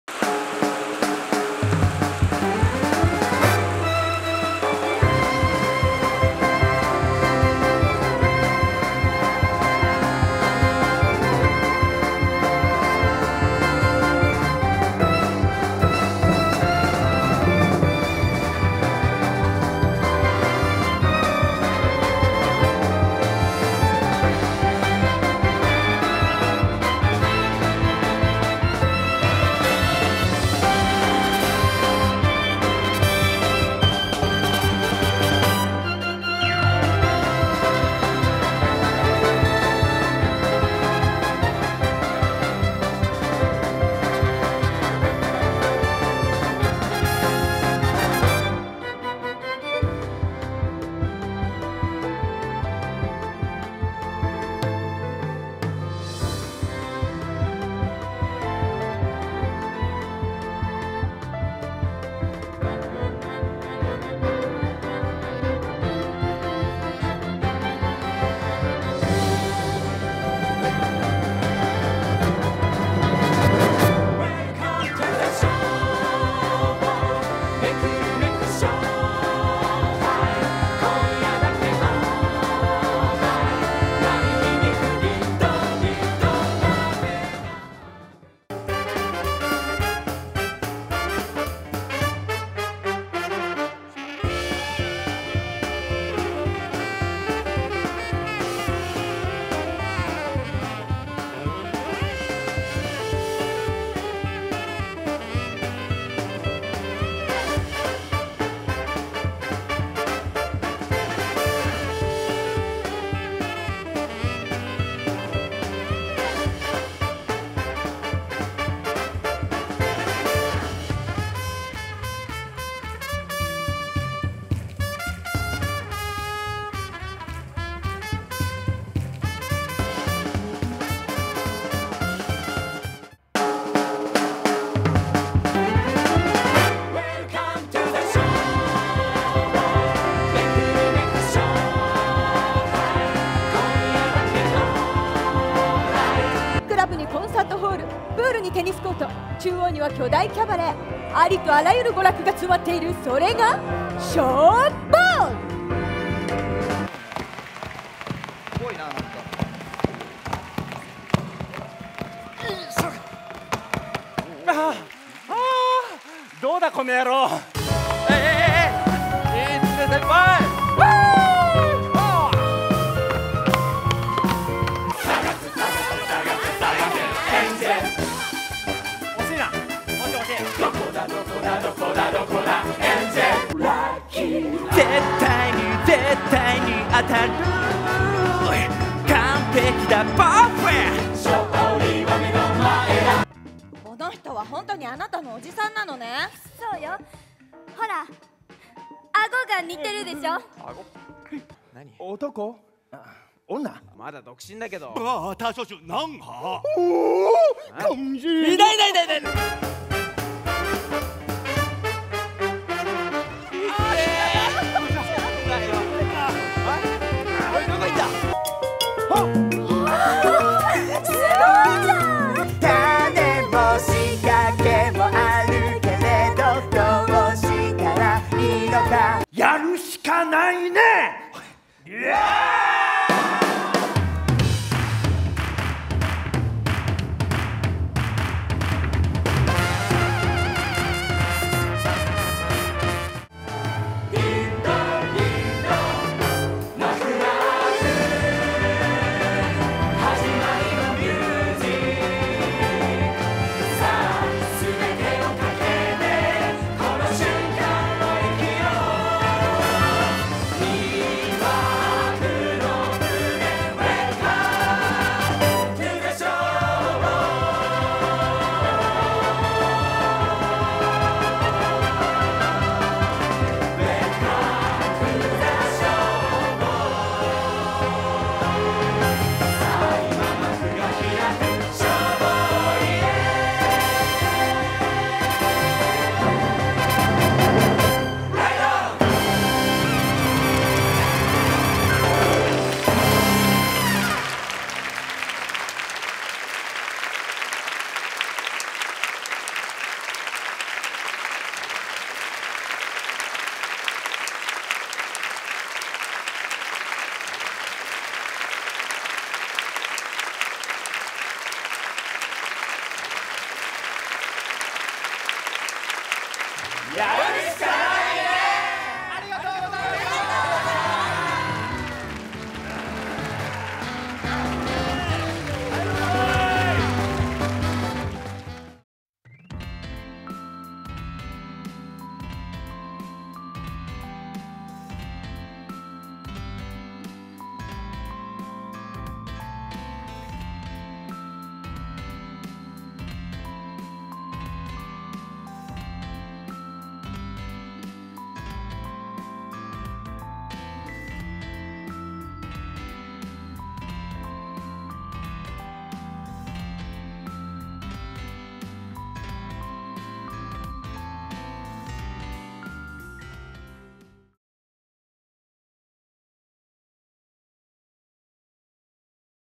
「SHOWBOY」名古屋公演（2021/8/7）ダイジェスト映像（J-LODlive2配信映像）
『SHOW BOY』
2021/8/7(SAT)  OPEN 13:00 START 12:00
2021/8/7(SAT)  OPEN 17:30 START 16:30
名古屋市公会堂 大ホール
NAGOYA CIVIC ASSEMBLY HALL

出演:福田悠太(ふぉ～ゆ～)
　　 辰巳雄大(ふぉ～ゆ～)
　　 越岡裕貴(ふぉ～ゆ～)
　　 松崎祐介(ふぉ～ゆ～)

　　 高田 翔
　　 高嶋菜七(東京パフォーマンスドール)
　　 辰巳智秋
　　 桐島十和子(PiXMiX)

　　 田口恵那/門間めい/遠藤 令/廣瀨水美/金川希美/高瀬雄史/東間一貴

　　 瀬下尚人(CONVOY)

　　 中川翔子

原案・演出:ウォーリー木下

製作:東宝